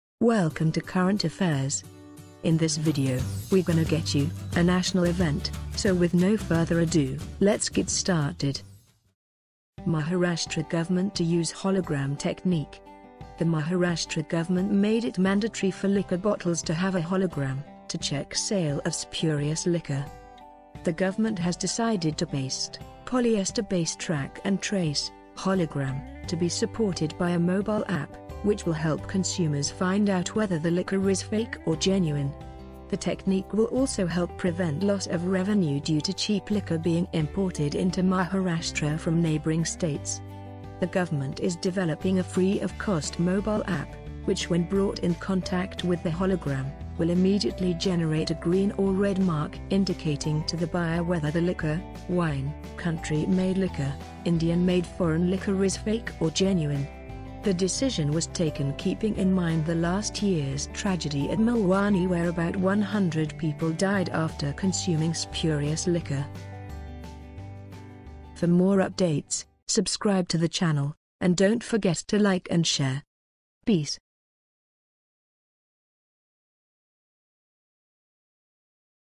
Current Affairs 20 April 2016 : Maharashtra Government to use hologram technique
The Maharashtra government made it mandatory for liquor bottles to have a hologram, to check sale of spurious liquor.